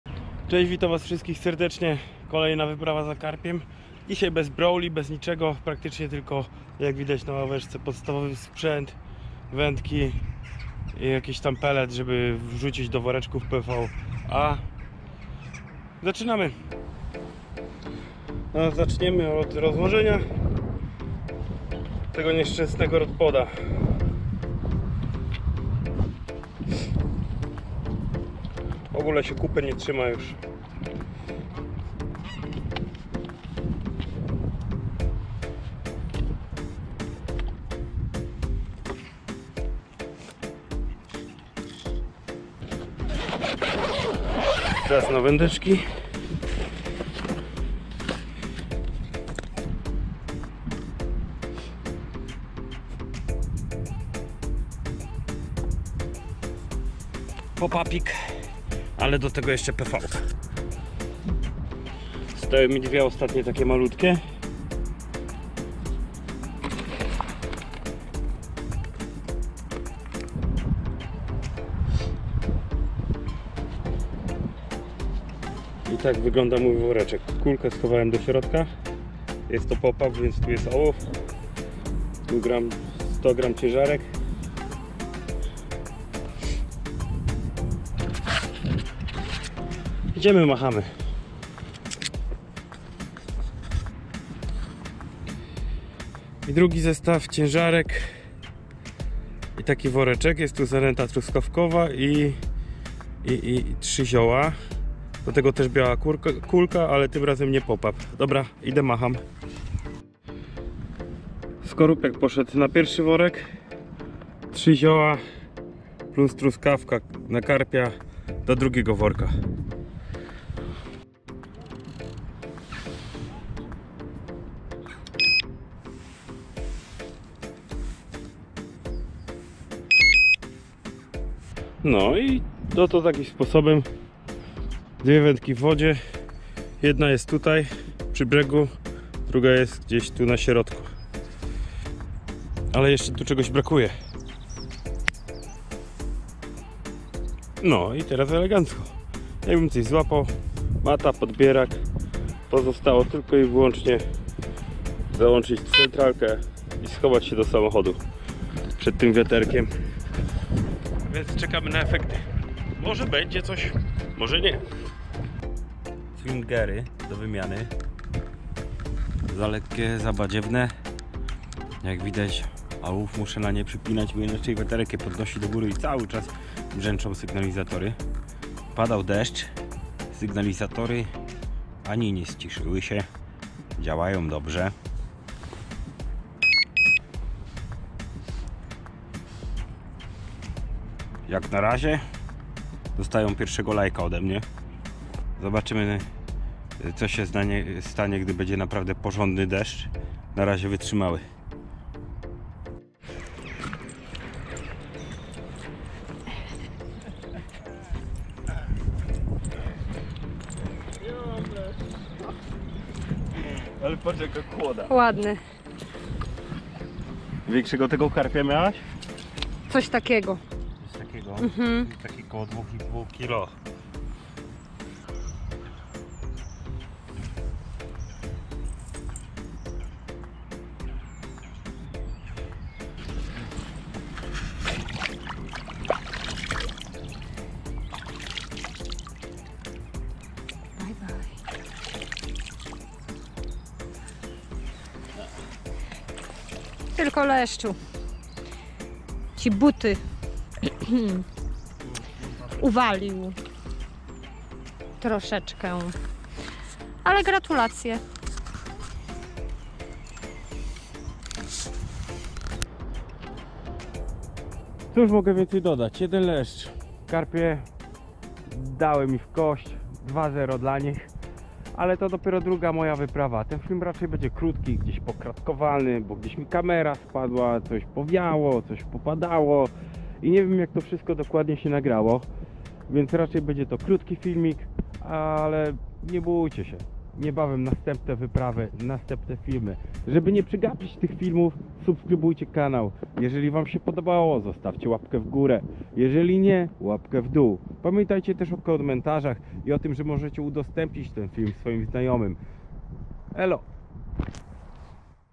2-0 Dla Karpi
Zapraszam serdecznie na kolejny film w ktorym probuje dobrac sie do karpi. Pozdrawiam